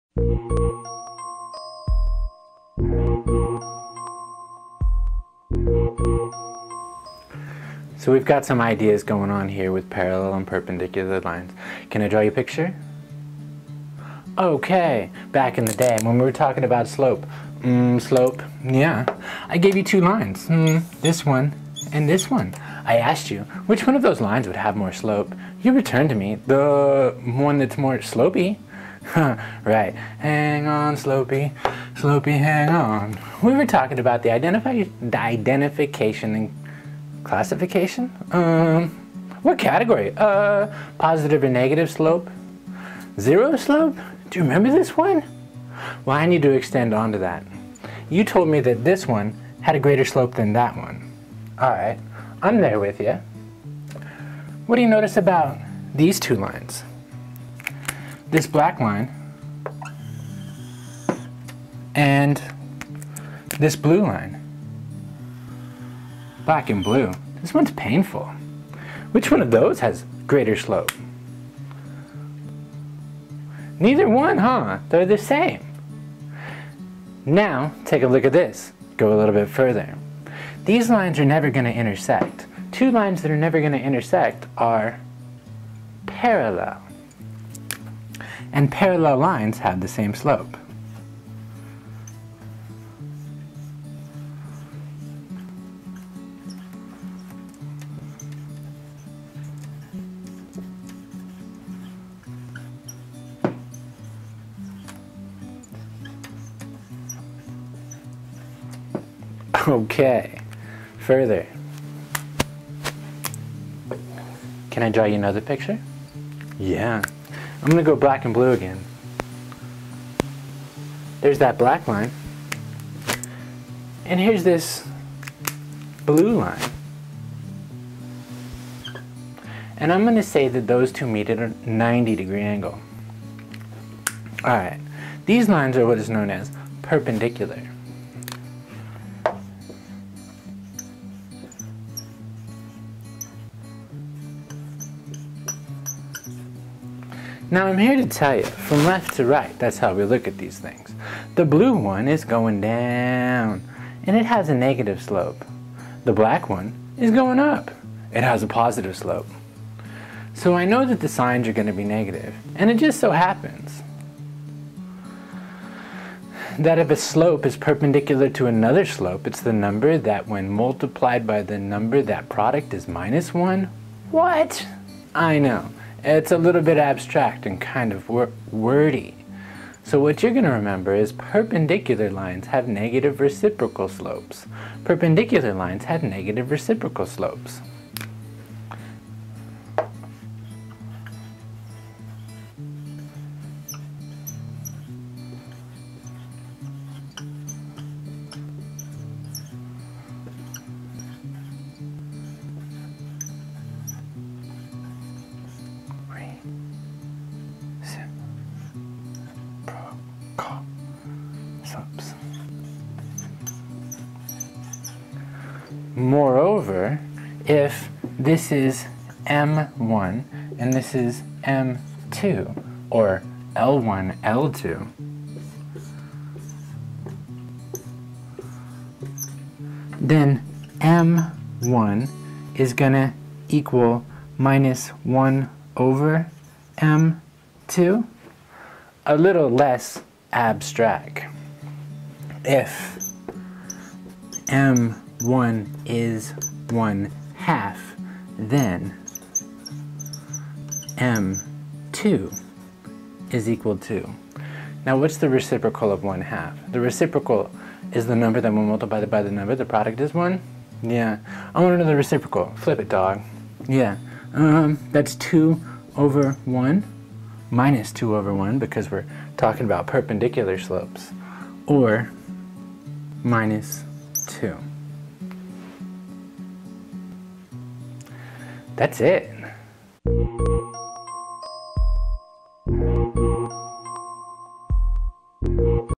parallel and perpendicular lines.mov
Parallel and perpendicular lines and the implications on slope. For more math shorts go to MathByFive.com